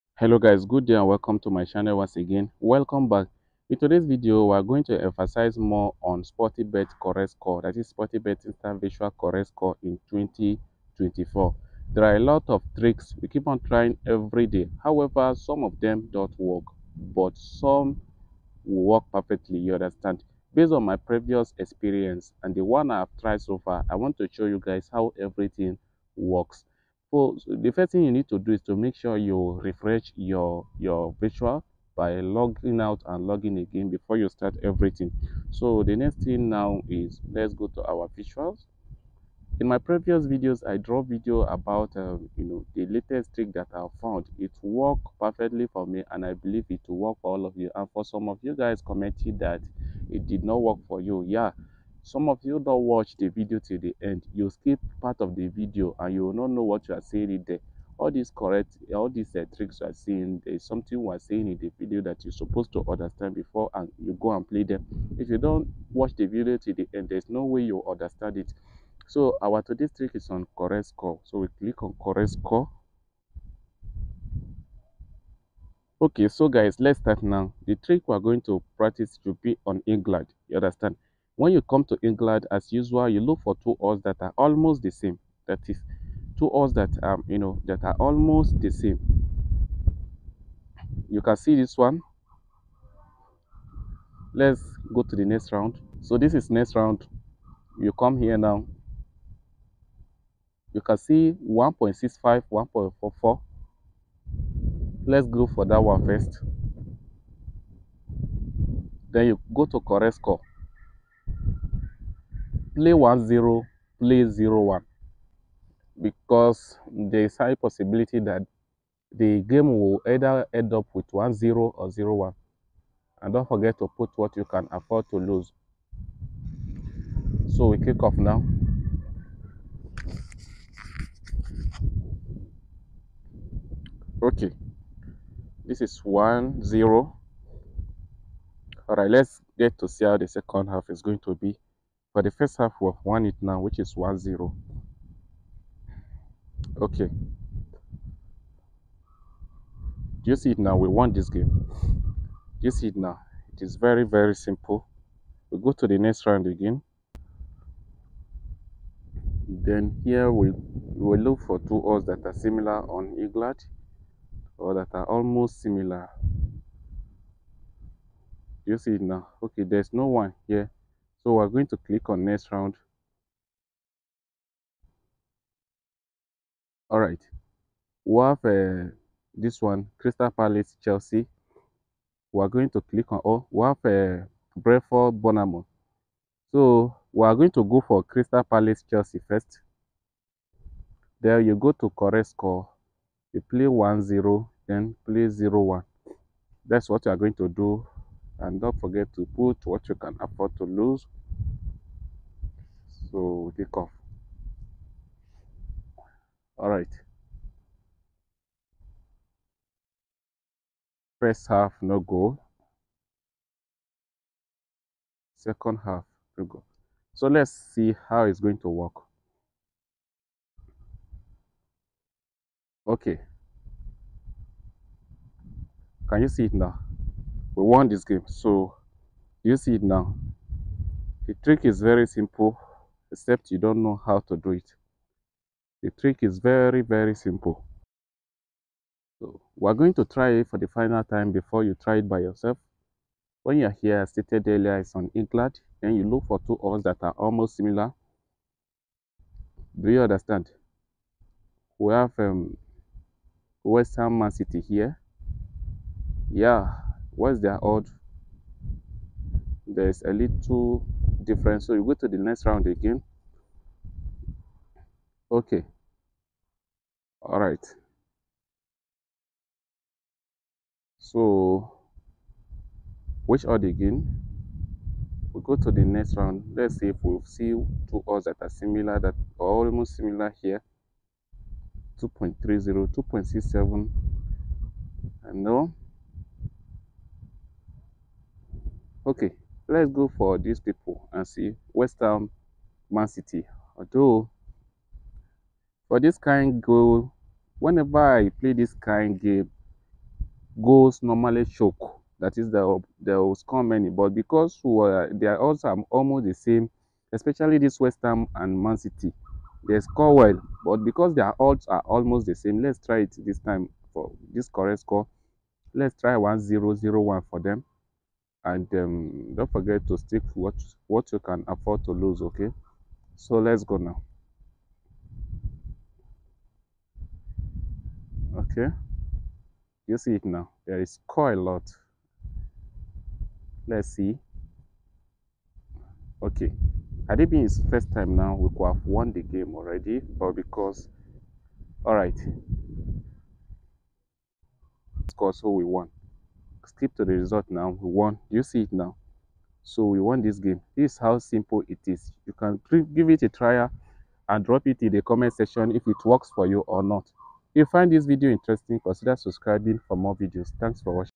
Sportybet Instant Virtual Correct Score Trick || Win Over 10.5 Odds✅💸
Sportybet Instant Virtual Correct Score Trick || New Correct Score Trick ✅💸

Welcome to the ultimate guide to mastering the latest correct score trick on Sportybet Instant Virtual! If you're looking to boost your winnings and elevate your virtual sports betting game, you've come to the right place. In this comprehensive video, we'll walk you through the step-by-step process of implementing this game-changing strategy that has personally yielded impressive results for me.

Discover the insider tips and tricks that can significantly enhance your success rate on Sportybet Instant Virtual. Whether you're a seasoned bettor or just getting started, this technique is designed to help you maximize your profits and minimize your losses.

In this tutorial, we'll delve into the following key aspects:
1. Understanding the dynamics of Sportybet Instant Virtual
2. Identifying patterns and trends for successful predictions
3. Implementing the correct score trick effectively
4. Managing your bankroll for long-term success
5. Tips for staying disciplined and focused during gameplay

Join me as I share my firsthand experience and insights into this groundbreaking strategy. With dedication, practice, and a bit of luck, you too can experience the thrill of consistently predicting correct scores and winning big on Sportybet Instant Virtual.

Don't miss out on this opportunity to take your virtual sports betting game to the next level.